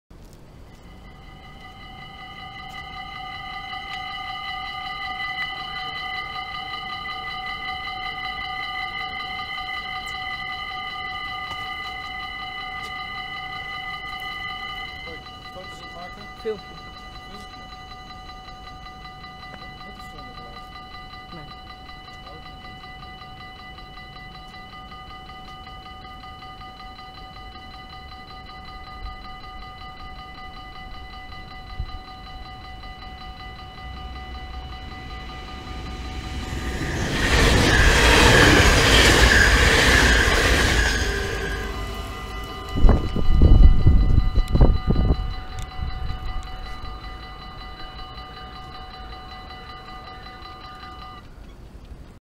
spoorwegovergang Driebergen-Zeist // Dutch railroad crossing // Bahnübergang
locasztie: odijkerweg, station Driebergen-Zeist
traject: utrecht - Arnhem, Veenendaal/Rhenen
rode lichten: 16
ontruimingslichten: 1
matrixborden: 1
bellen: 7
bomen: 6
andreaskruisen: 5
soort: AHOB
passeren:
-VIRM als IC Den Helder→Nijmegen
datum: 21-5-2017